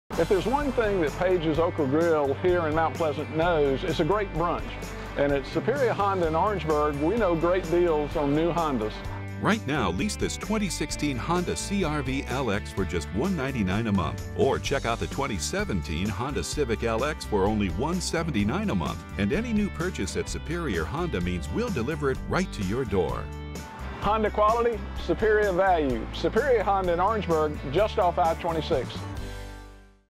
Superior Honda TV Commercial at Pages Okra Grill featuring Jimmy Guthrie
Jimmy Guthrie of Superior Honda Orangeburg, SC TV commercial at Pages Okra Grill in Mt. Pleasant.